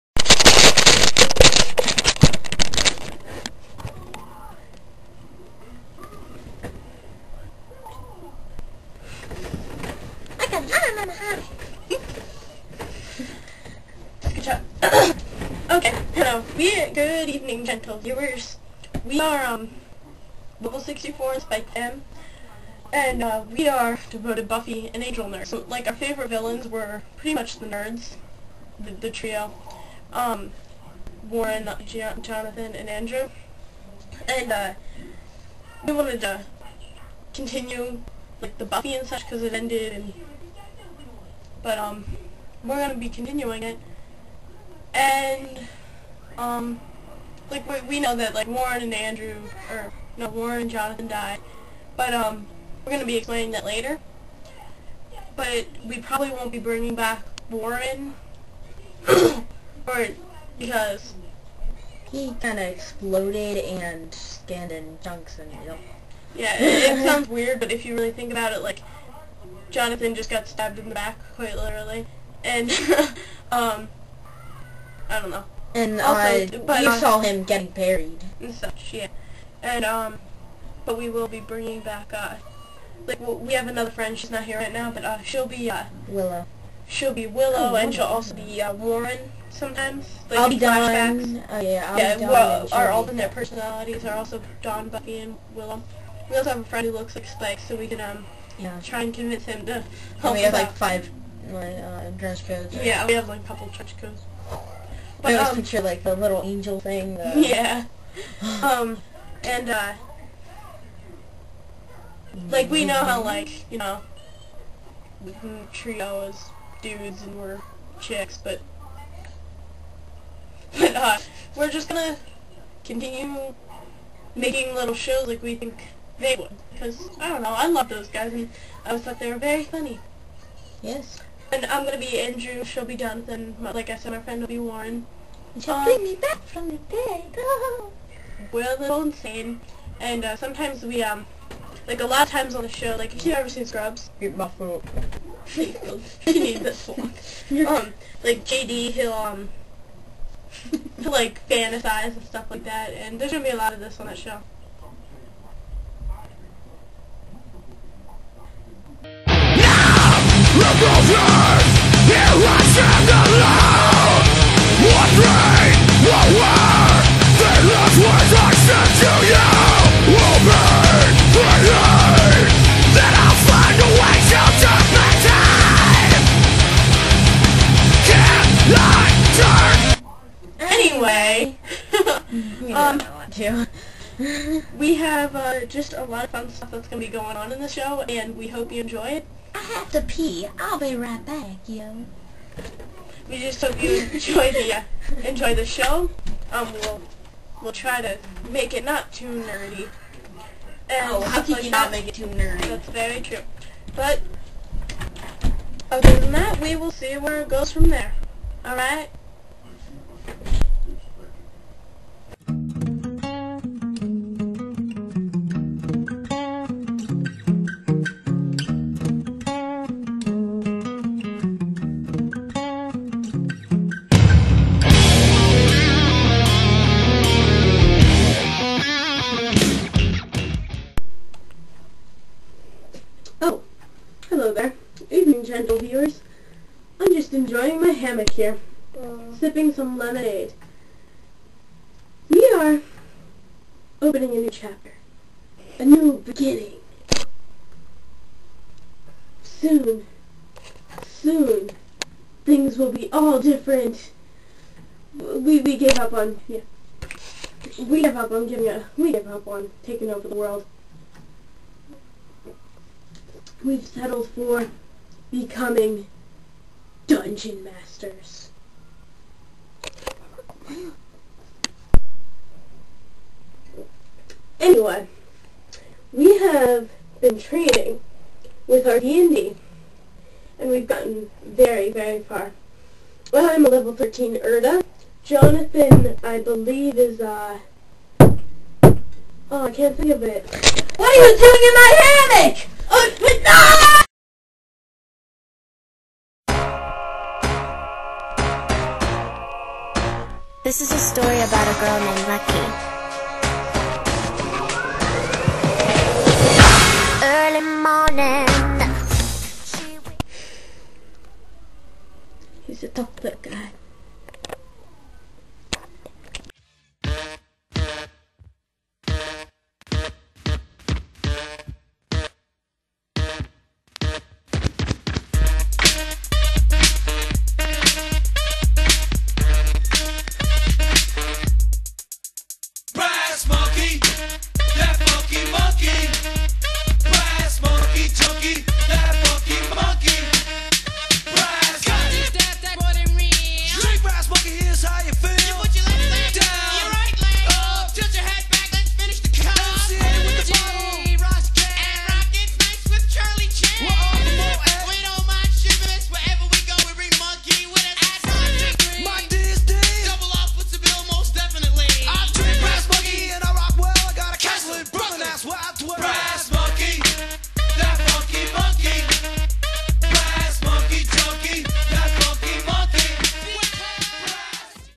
The Adventures of Johnathan and Andrew
Greetings gentle veiwers! This is our first video in the series of Johnathan and Andrew! WE LOVESES DA BUFFY!! WE LOVESES YOOOUUU!!! Anyway, enjoy!! (Oh and cuz this is the first episode it kinda fails...later ones will be better!!)